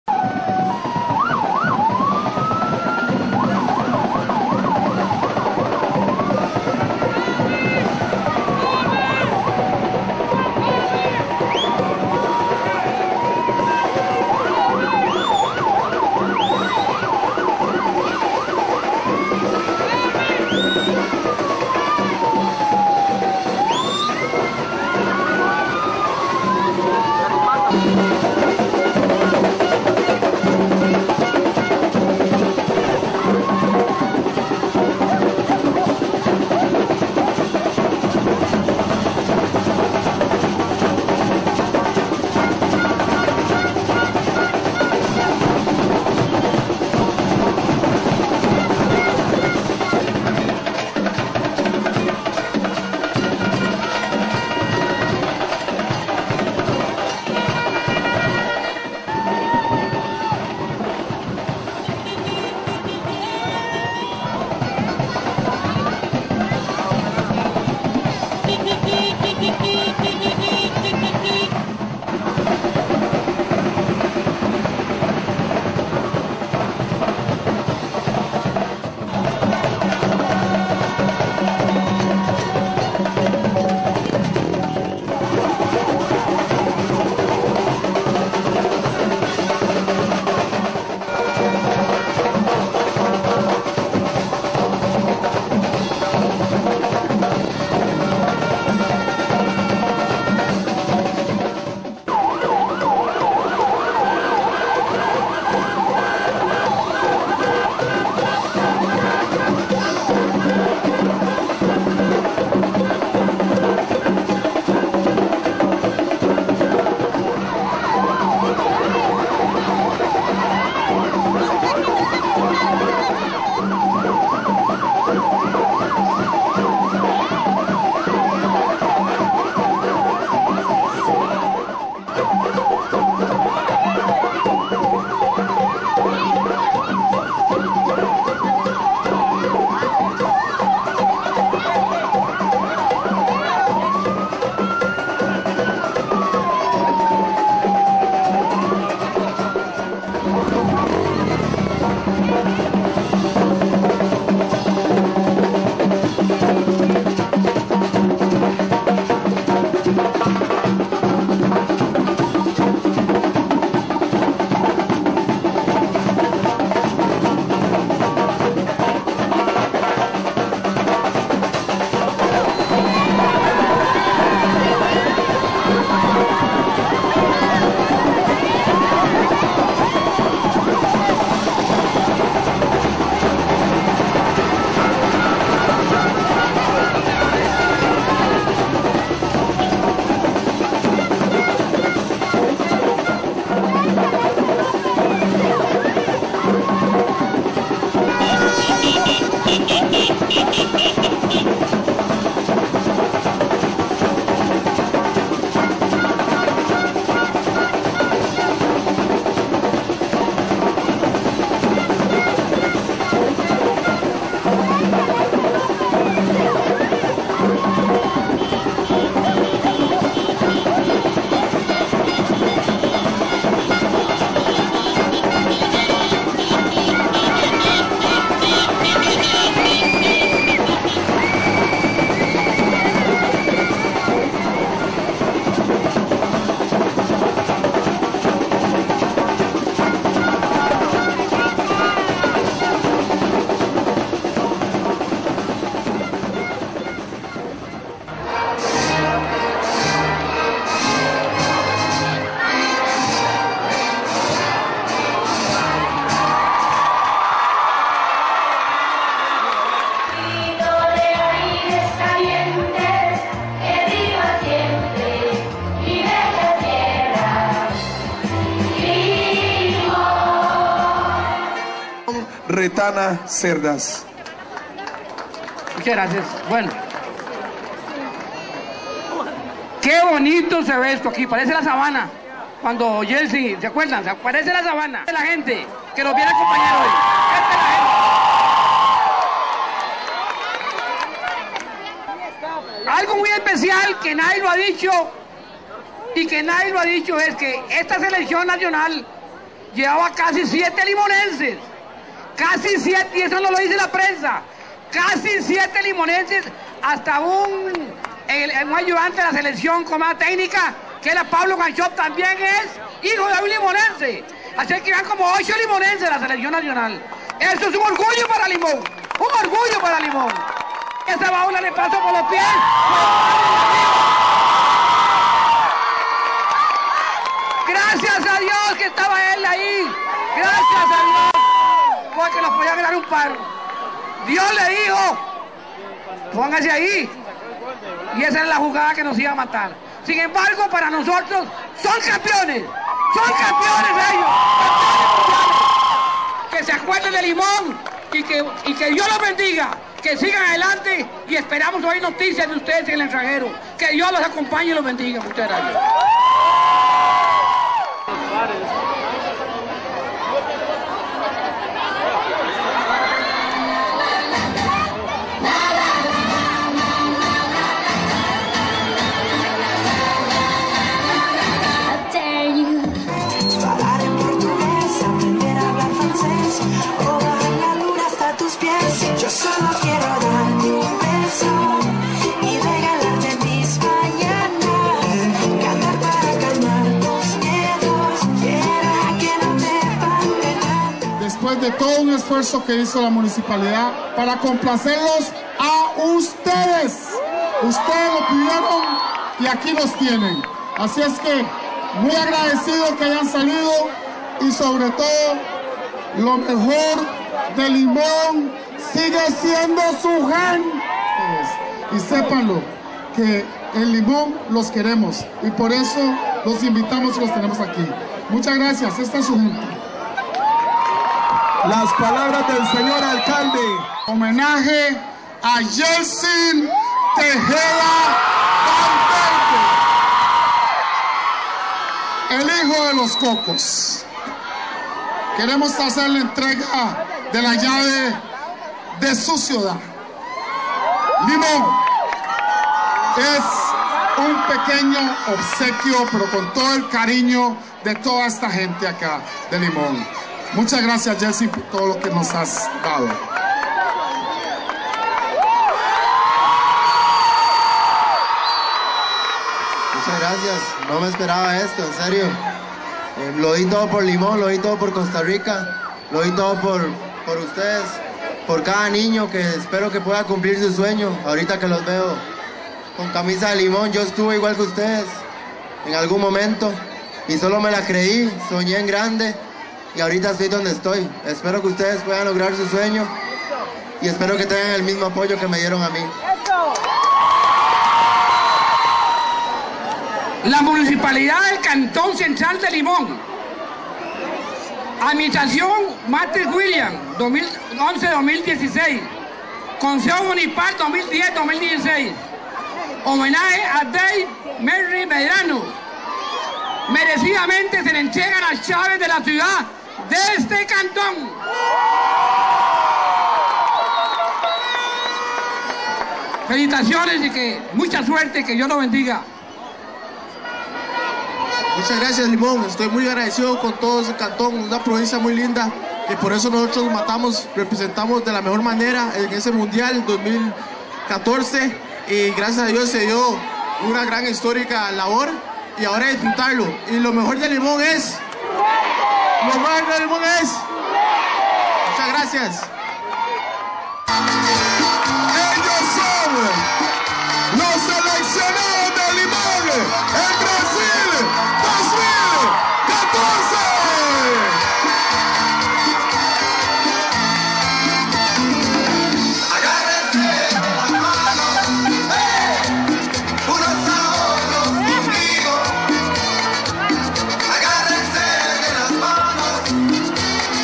Caravana de bienvenida a seleccionados Limonensew
Caravana de bienvenida y homenaje a seleccionados Limonenses, YELTSIN TEJEDA Y DAVE MYRIE.